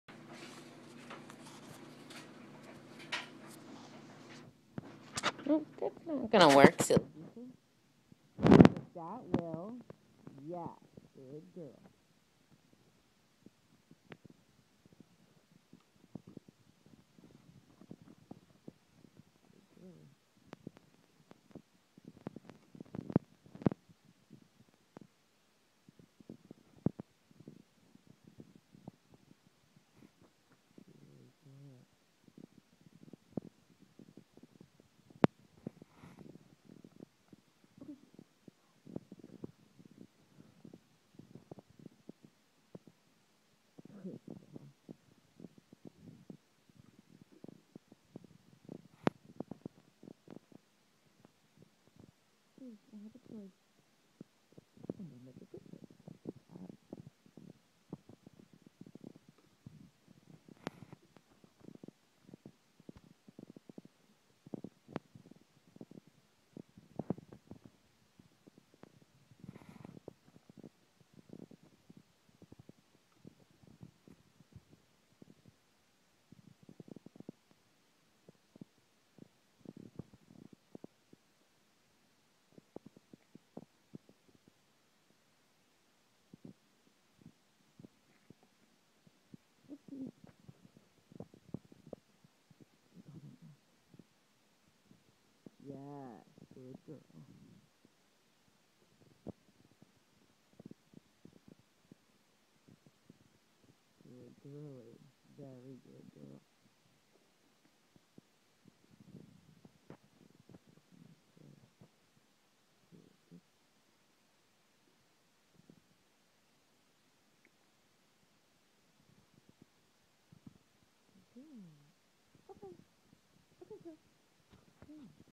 Teaching "go to mat"
Teaching the baby bc go to mat, preparing to incorporate manners minders. Good girl